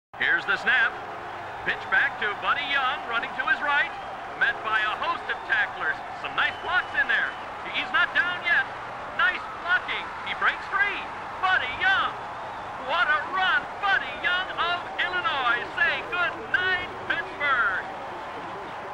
Buddy Young run vs pitt
Buddy Young scores a long running touchdown against Univeristy of Pittsburg.